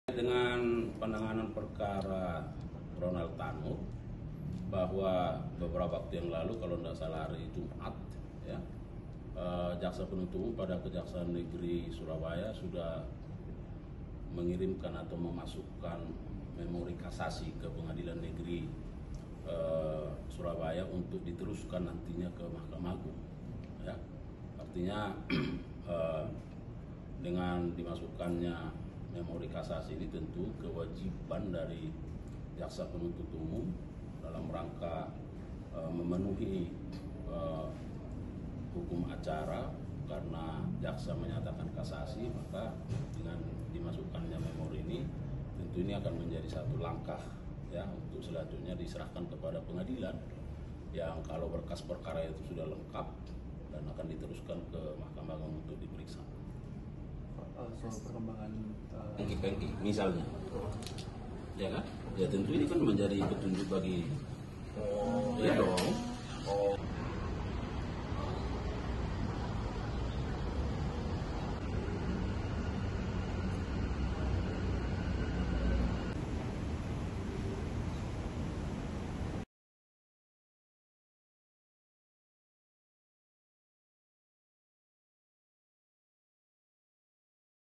Kabar Terbaru Soal Putusan Bebas Ronald Tannur, Jaksa Layangkan Memori Kasasi
Jaksa Penuntut Umum (JPU) Kejaksaan Negeri (Kejari) Surabaya disebut telah melayangkan memori kasasi ke Pengadilan Negeri Surabaya, terkait vonis bebas Gregorius Ronald Tannur.

Kepala Pusat Penerangan Hukum (Kapuspenkum) Kejaksaan Agung, Harli Siregar mengatakan, kasasi tersebut telah dilayangkan oleh pihak JPU pada Jumat (16/8/2024) lalu.